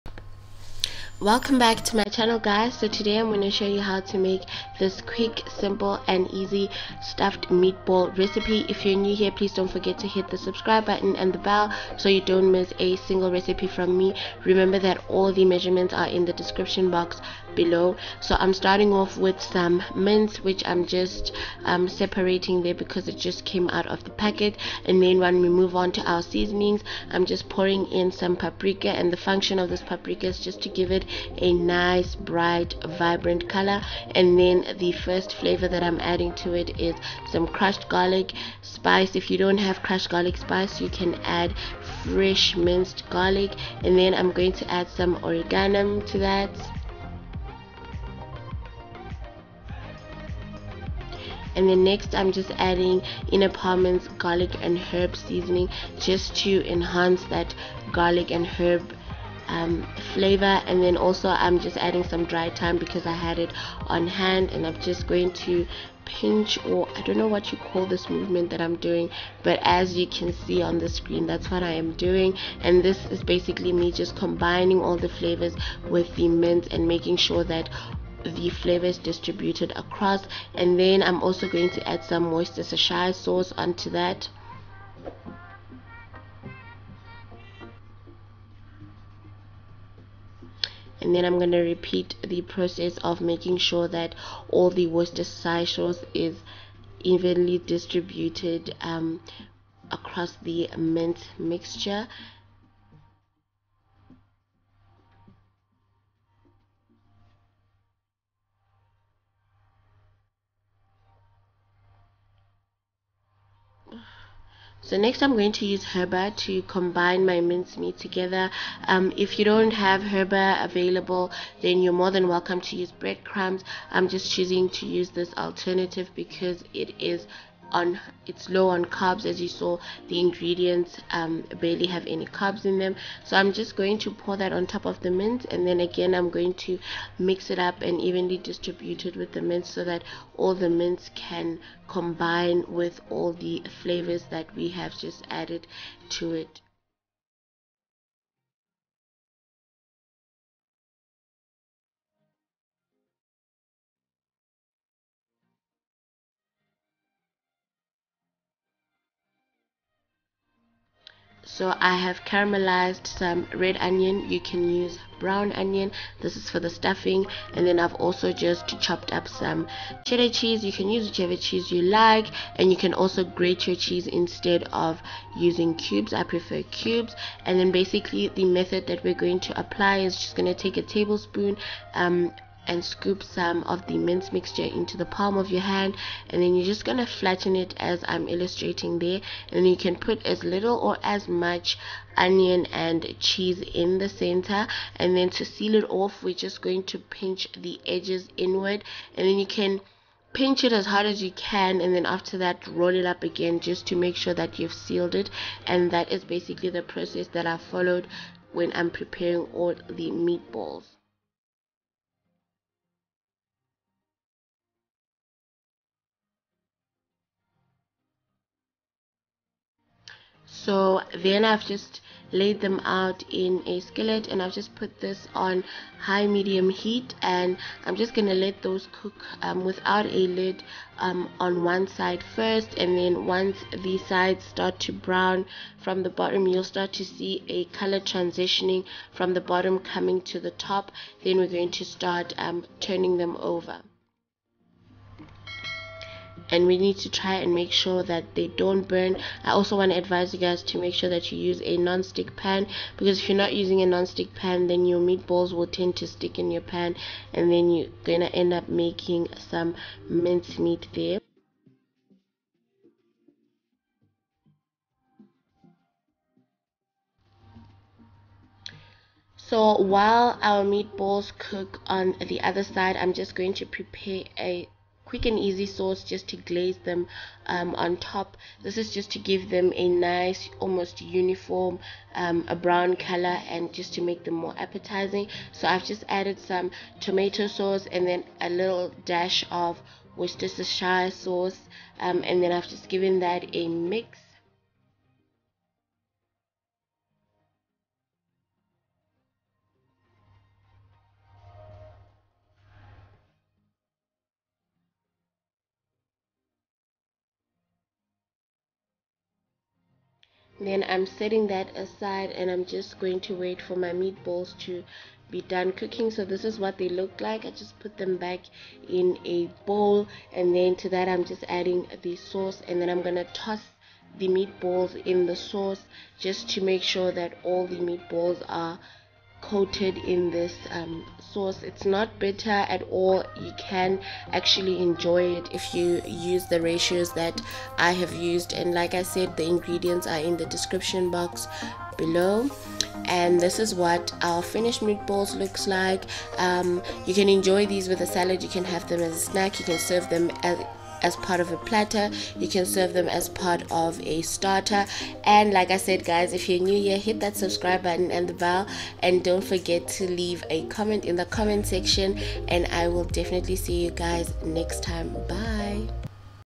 Cheese and Onion Stuffed Meatballs
Hi guys

Ingredients :

600g need mince
1 tablespoon  crushed garlic
2 tablespoons paprika
1 tablespoon  garlic and herb spice
1 table spoon  oregano
1 teaspoon thyme
2 tablespoons heba
1 tablespoon Worcestershire sauce

Stuffing :
1/3 Onion  diced (cooked)
Cheddar cheese cubes/grated

Sauce

3 table spoon tomato sauce
1 teaspoon Worcestershire sauce

PObox 6208
Onverwacht
0557